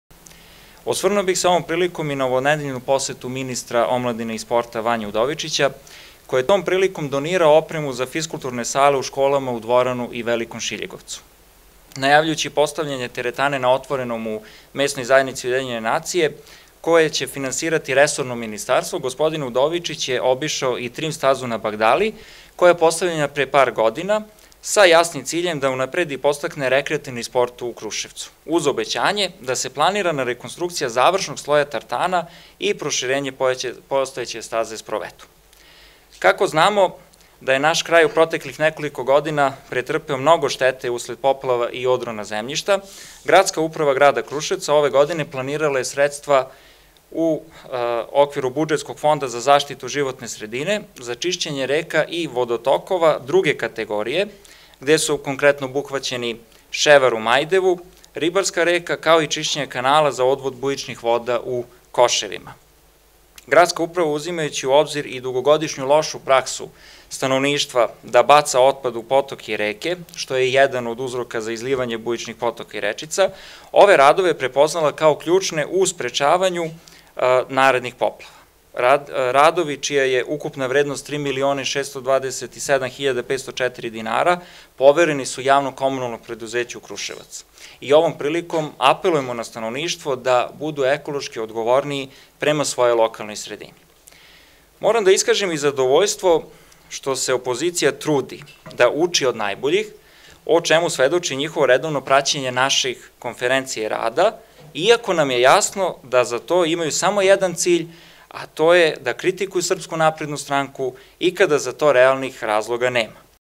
SNS KRUSEVAC: OPOZICIJA NAS PRATI I UCI OD NAJBOLJIH
U prostorijama Srpske napredne stranke u Kruševcu 31. oktobra 2018. godine, održana je redovna konferencija za medije. Na konferenciji je govorio Zoran Tomić, član Saveta za informisanje.